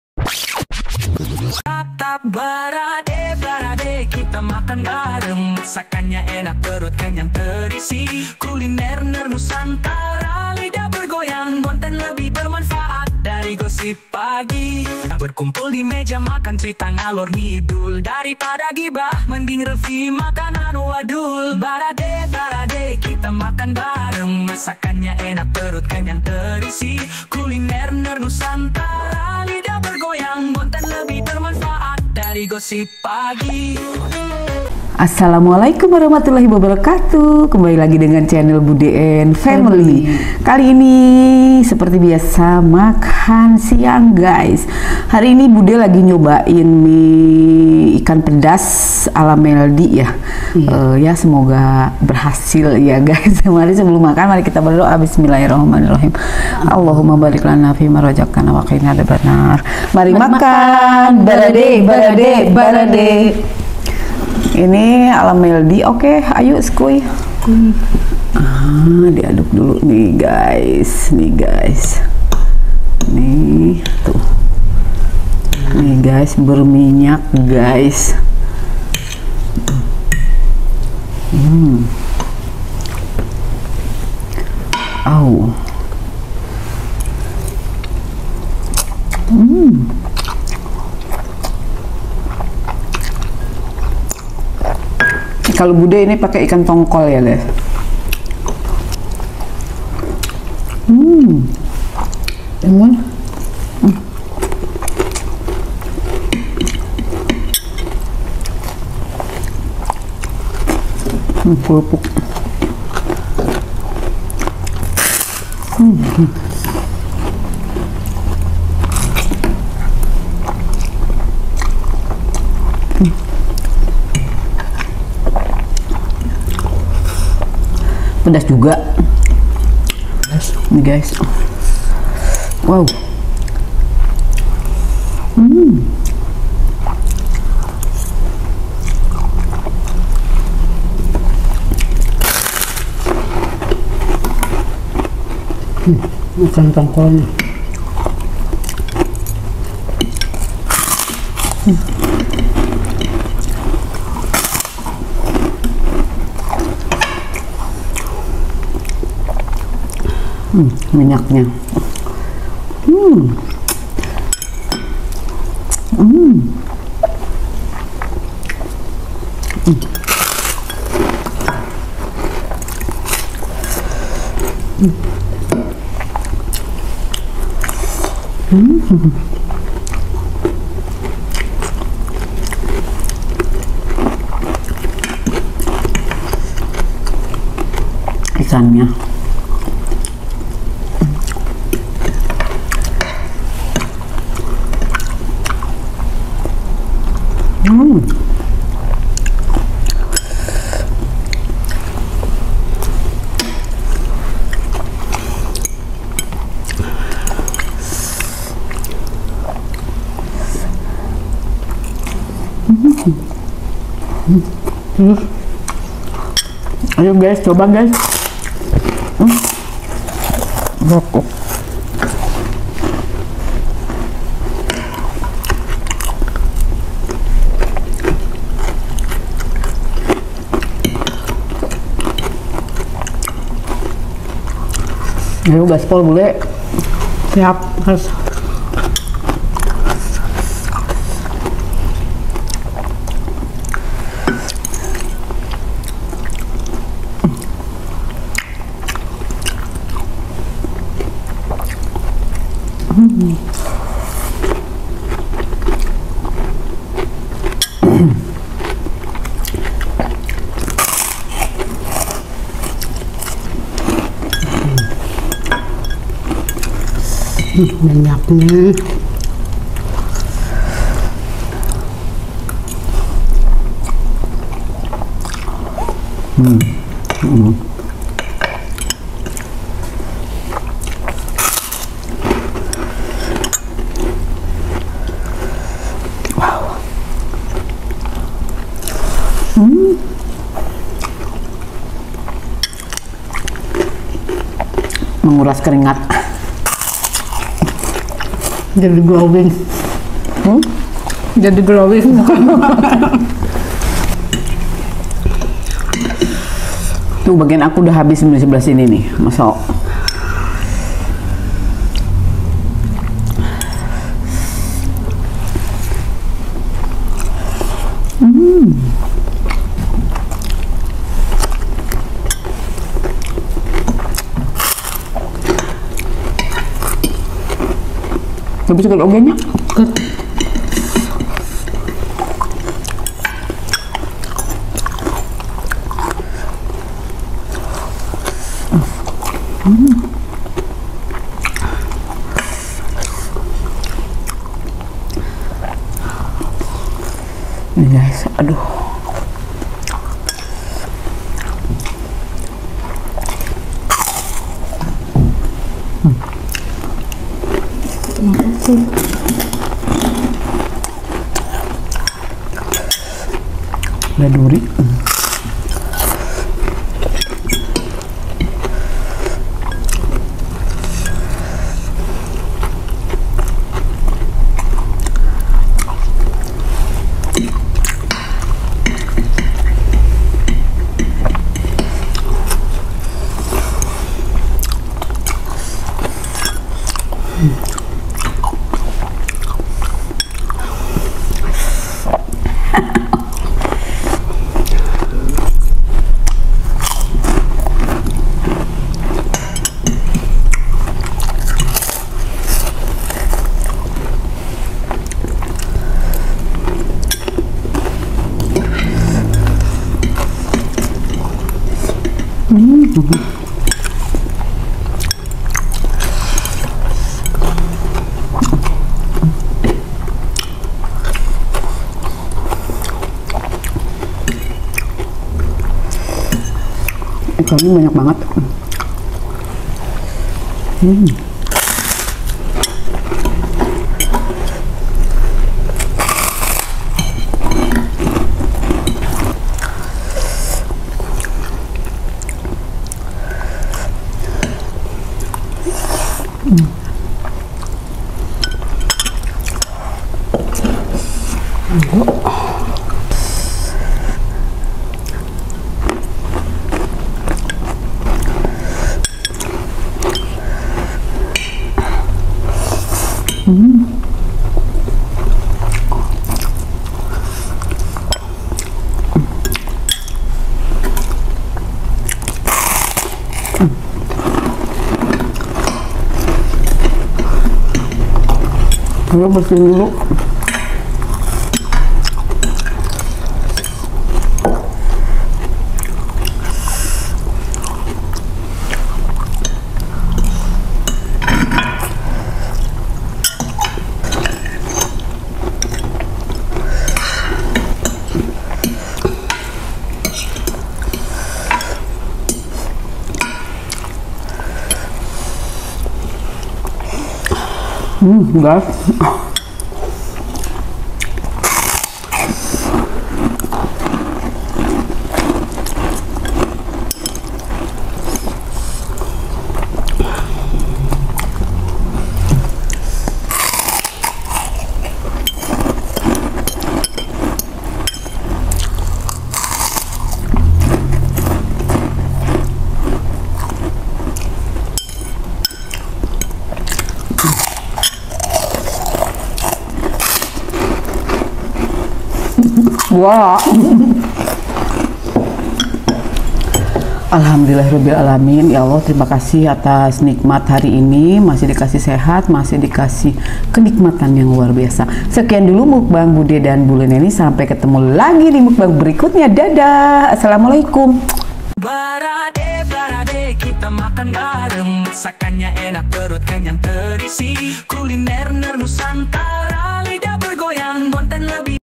PEDAS SADIS‼️MIE PEDAS IKAN TONGKOL SADIS ENAKNYA🔥🔥
Hari ini budhe dan bulek mukbang mie pedas ikan tongkol ala meldi rasanya nampol enak pake banget,semoga terhibur enjooy❤️❤️